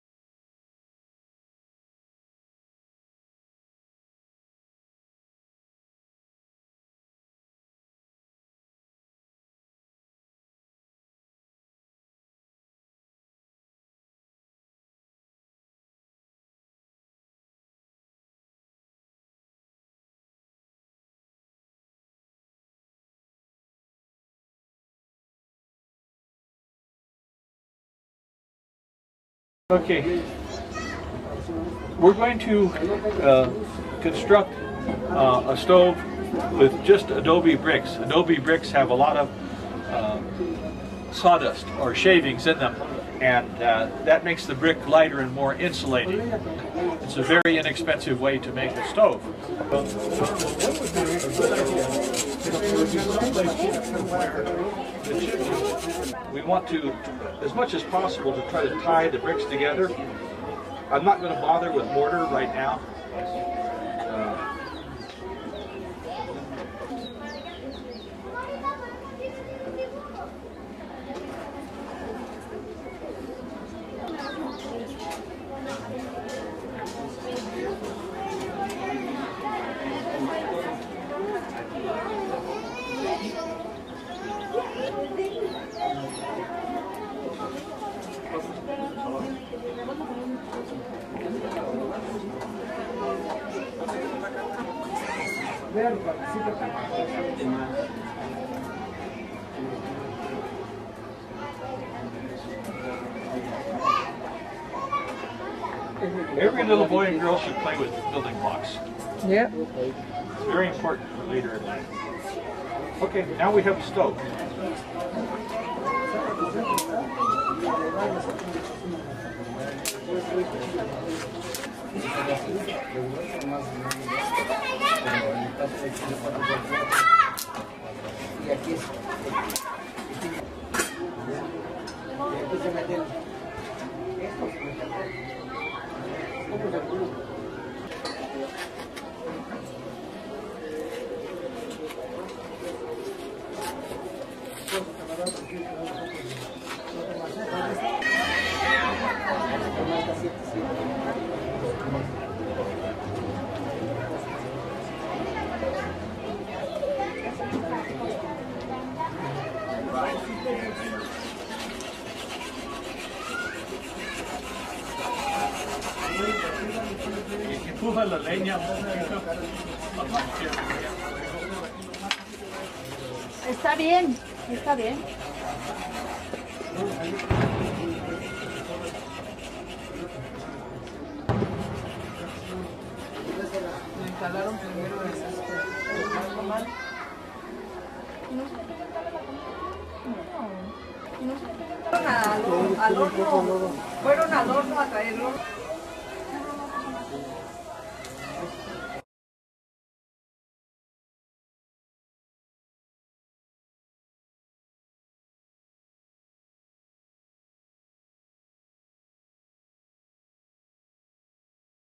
How to Make a 16 Brick Rocket Stove
Dr. Larry Winiarski makes a clean burning rocket stove using 16 adobe bricks at the Rotary International-sponsored Integrated Cooking Workshop in Tlautla, Mexico